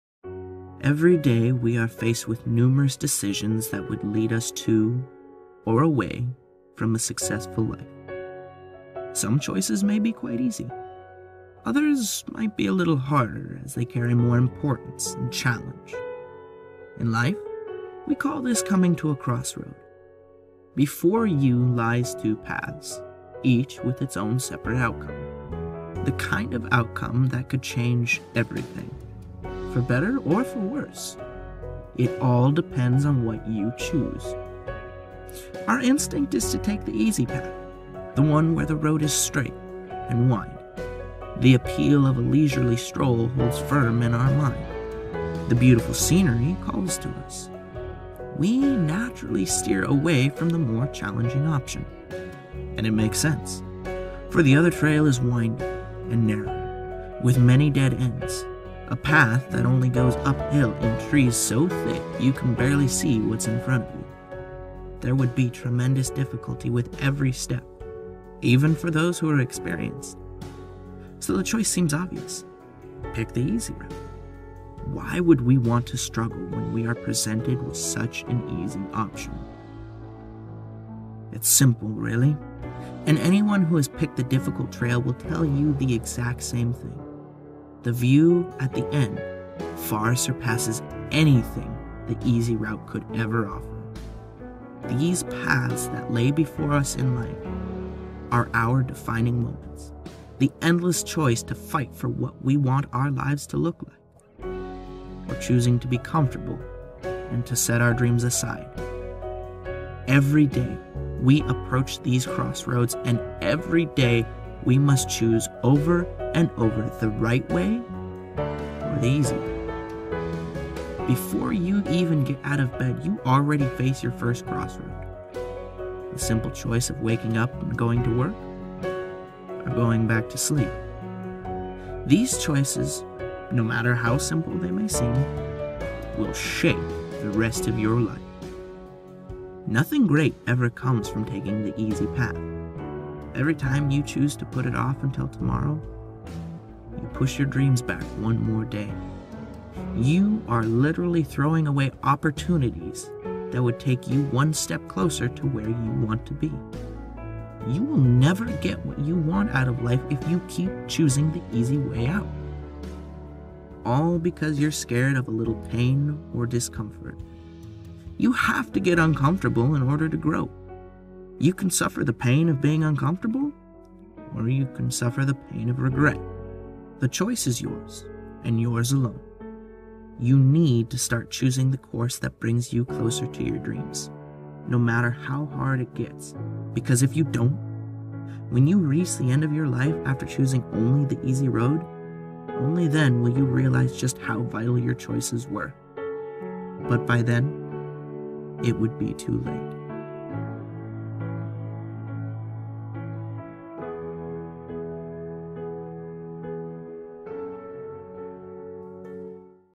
Crossroads - MOTIVATIONAL VIDEO
We are faced with many choices in our life. The question is, which path will you choose?